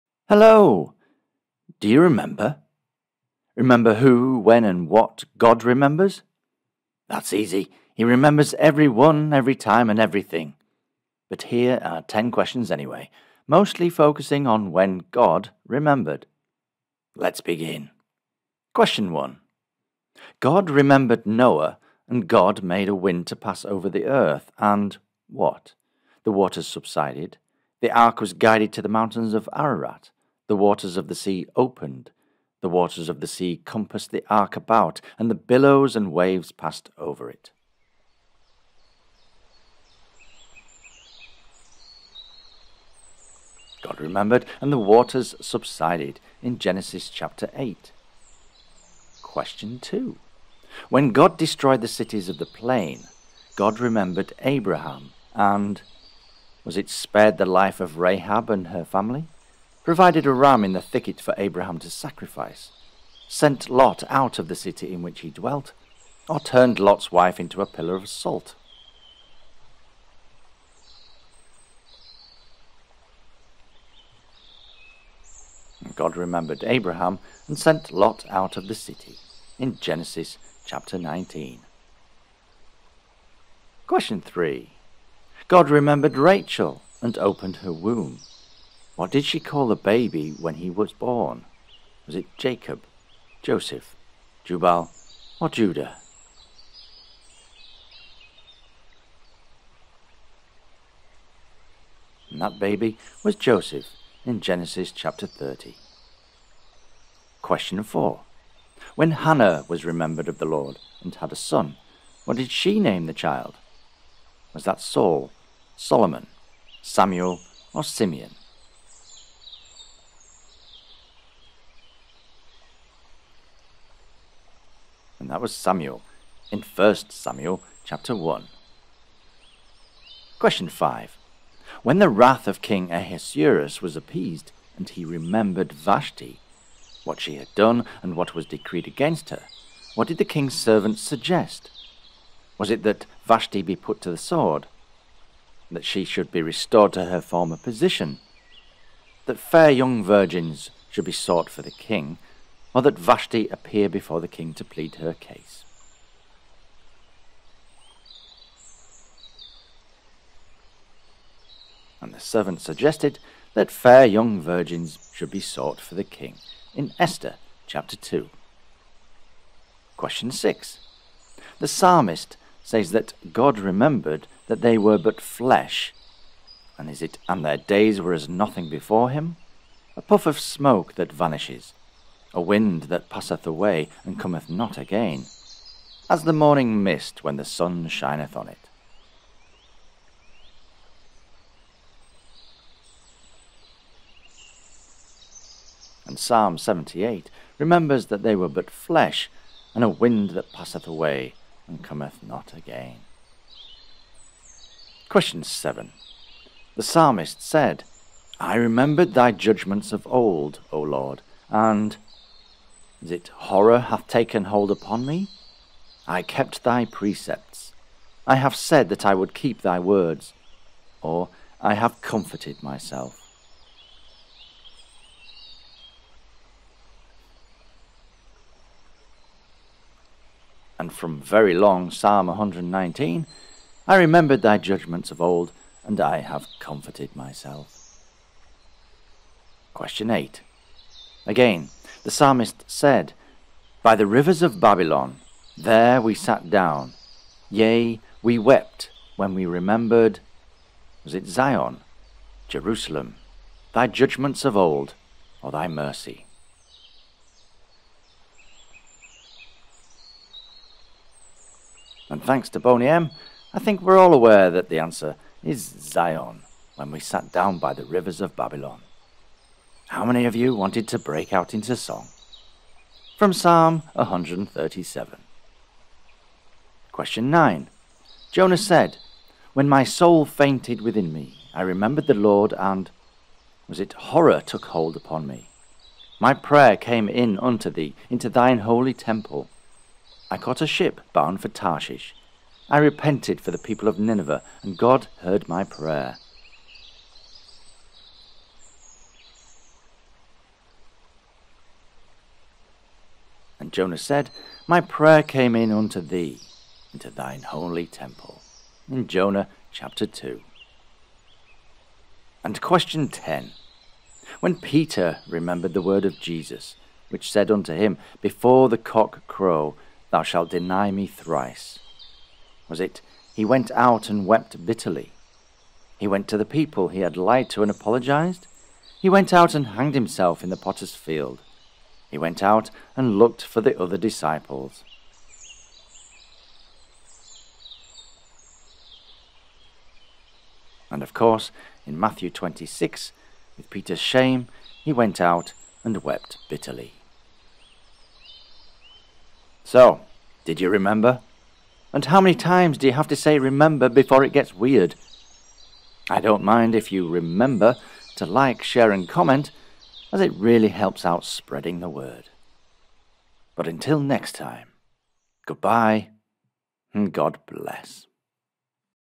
Bible Quiz | How Many of These Themed Trivia Questions Will You Know?
10 questions from around the Bible.